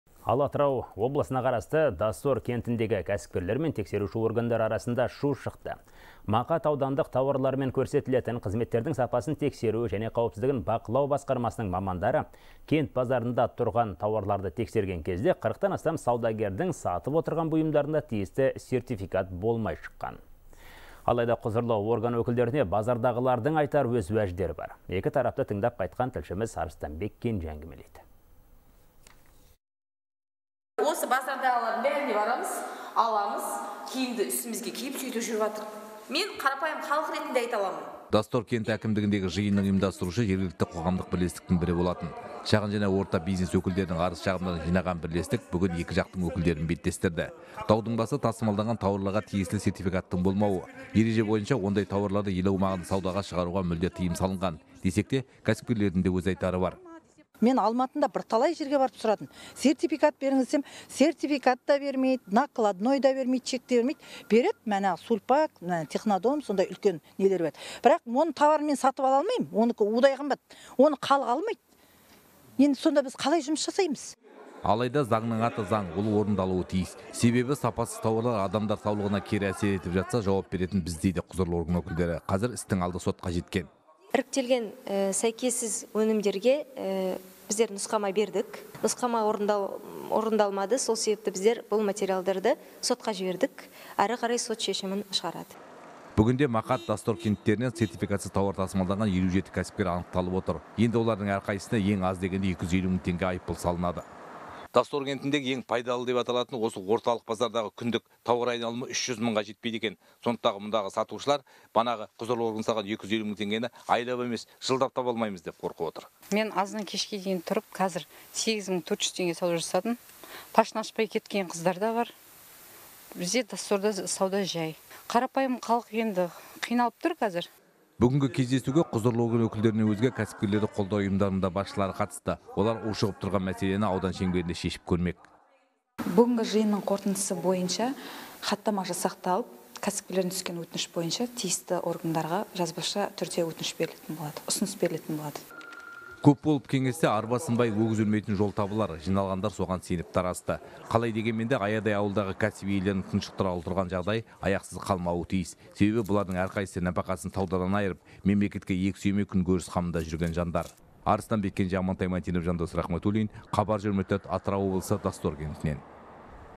Доссор кентінде тексеруші органдар мен кәсіпкерлер арасында дау шықты
Атырау облысына қарасты Доссор кентіндегі кәсіпкерлер мен тексеруші органдар арасында шу шықты. Мақат аудандық тауарлар мен көрсетілетін қызметтердің сапасын тексеру және қауіпсіздігін бақылау басқармасының мамандары кент базарында тұрған тауарларды тексерген кезде 40-астам саудагердің сатып отырған бұйымдарында тиісті сертификат болмай шыққан.